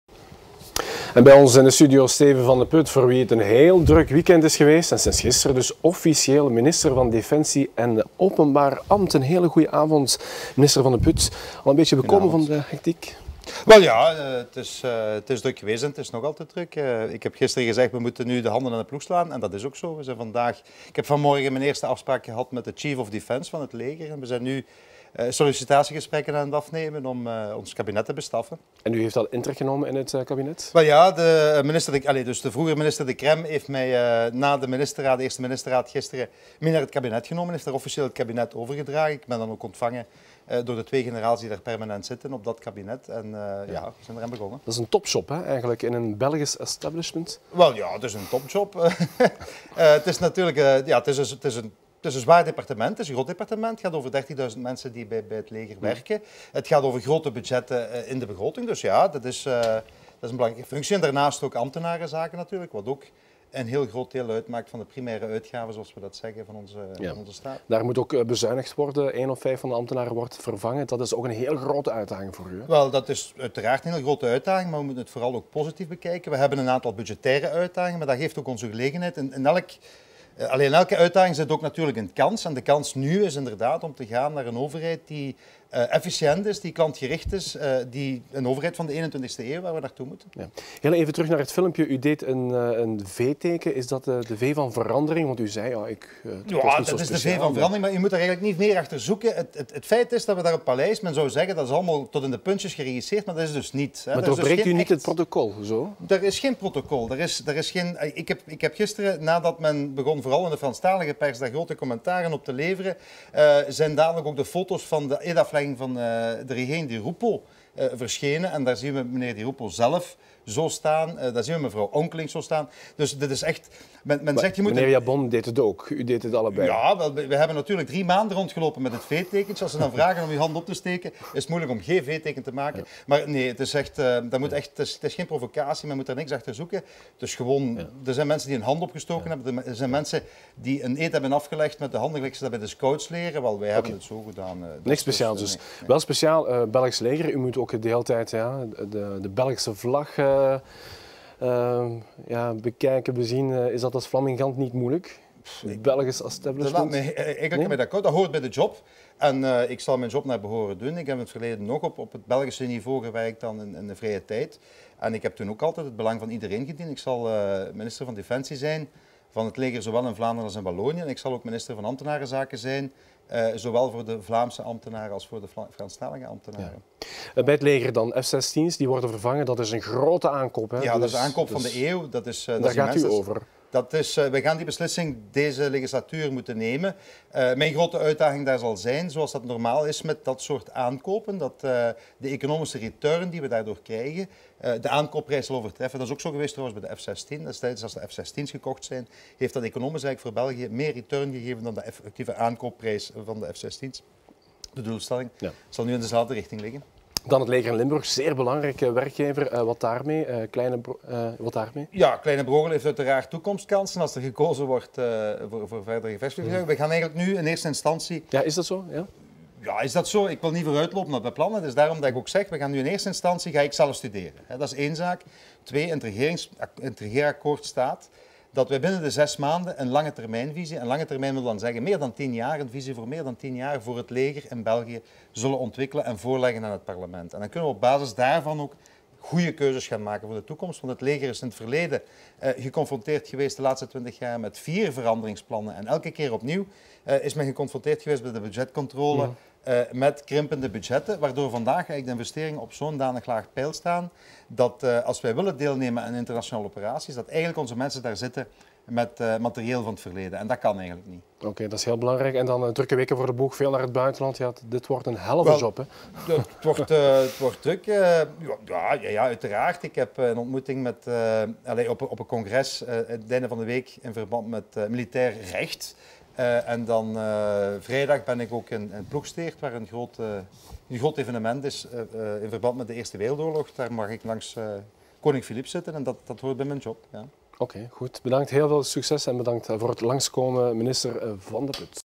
Minister Steven Vandeput: 'Ik zal minister zijn van Vlamingen en Walen'
De kersverse minister van defensie en openbaar ambt Steven Vandeput was vanavond bij ons te gast in de studio.